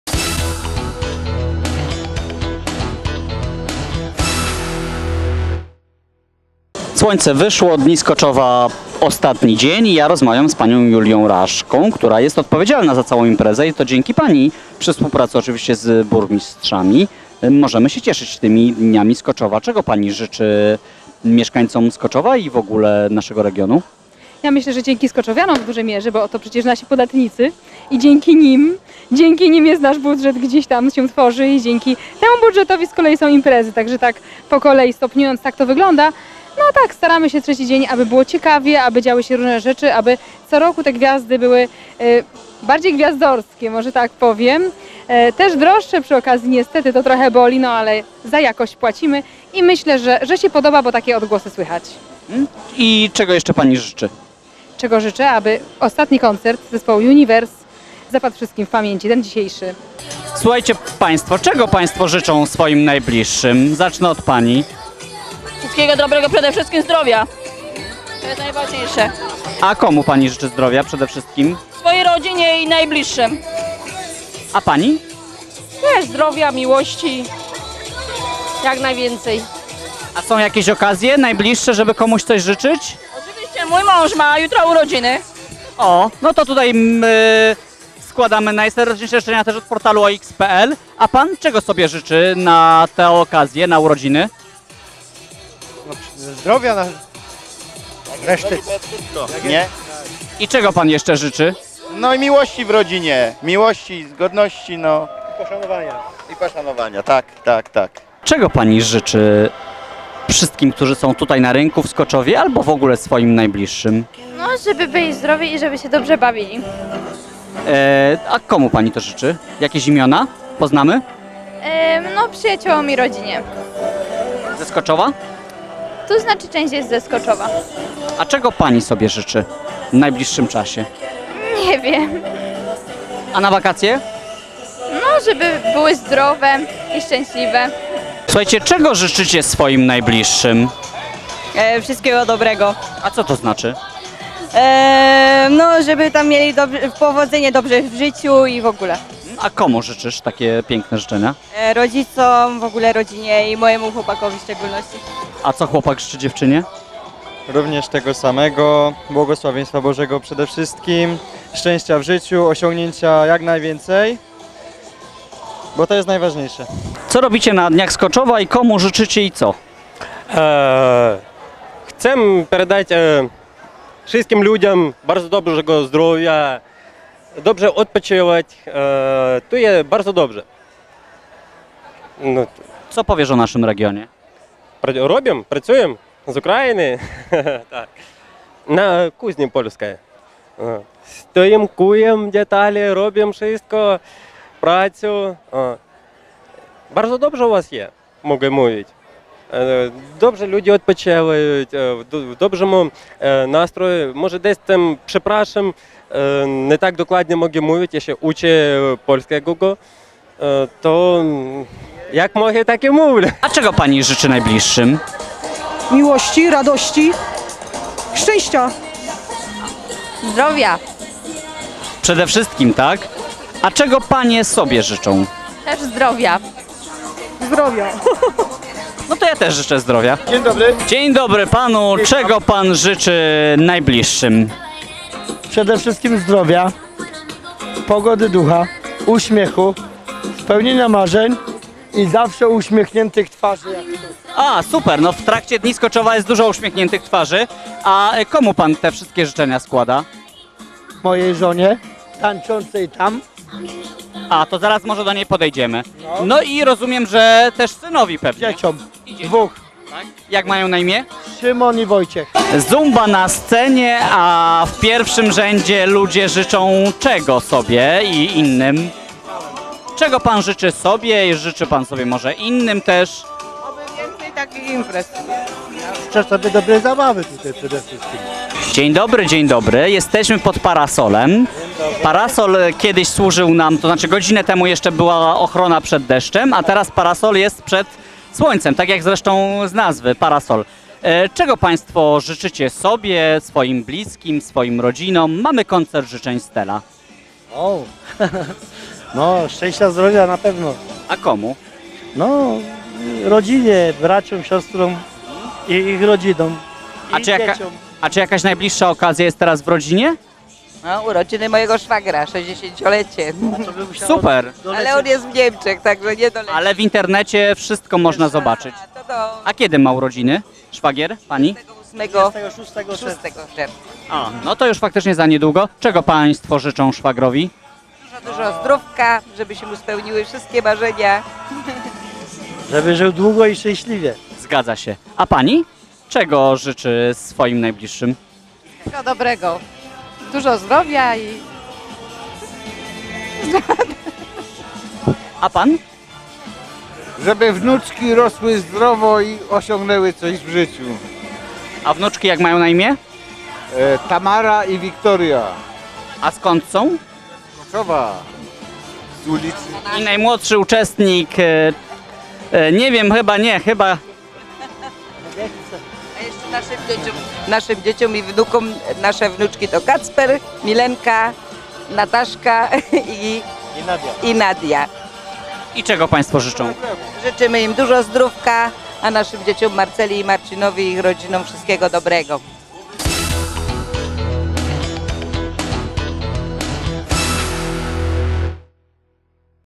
Koncert Życzeń Stela - Na żywo z 51 Dni Skoczowa BONUS cz. 2
┌ Rozwiń Opis

Dziękujemy Wam za wszystkie nadesłane życzenia i zapraszamy na kolejny odcinek naszego cyklu ''Koncert Życzeń Stela'' w Portalu Śląska Cieszyńskiego OX.PL.

Program poprowadzi